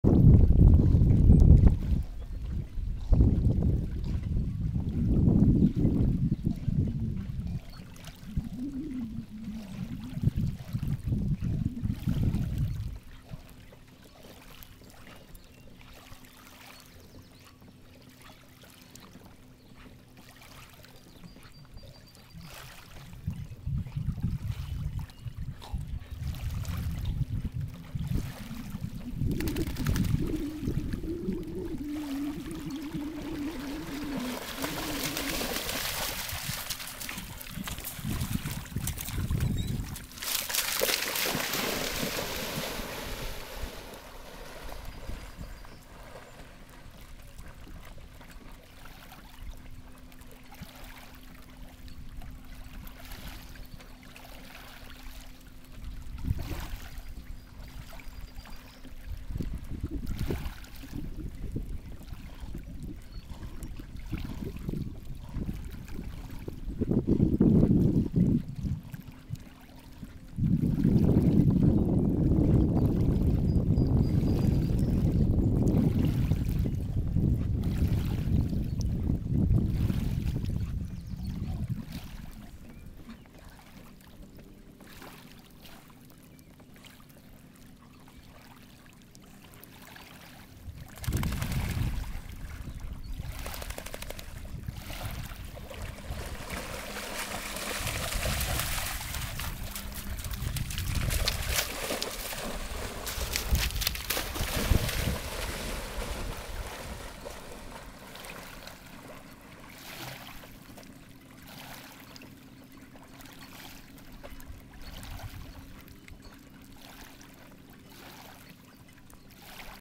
Swimming in the Lake Black Lab and Plott Hound Part 2
Swimming in Lake Lemon Memorial Day Weekend 2008